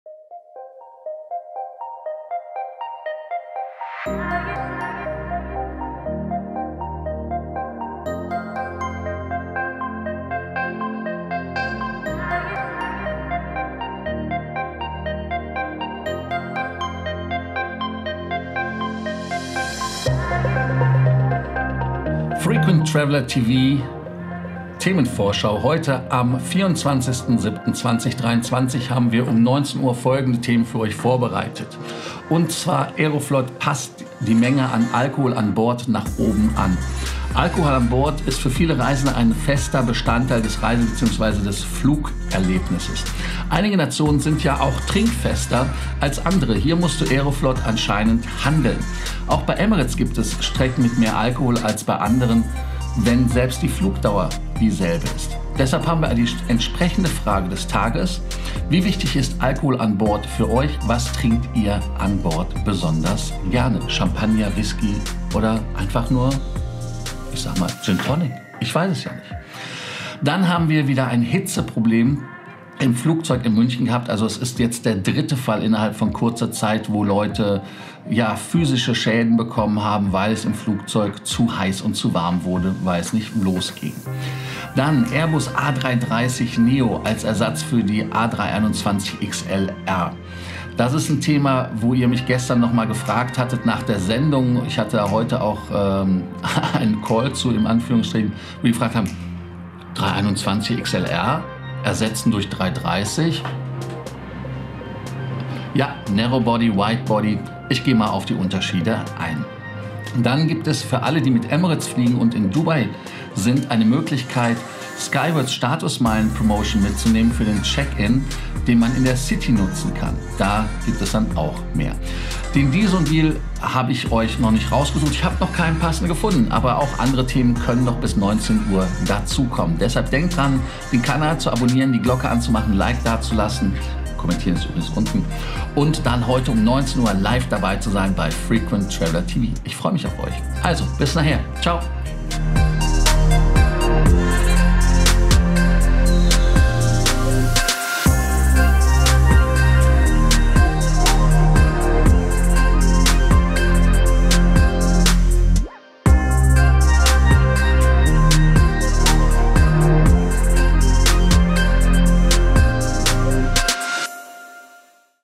Themenvorschau: ✈️⬆️ Aeroflot passt die Menge an Alkohol an Board nach oben an
Die Themenvorschau auf die heutige Sendung um 18 Uhr deutscher Zeit.

Clubhouse - @LFCorsten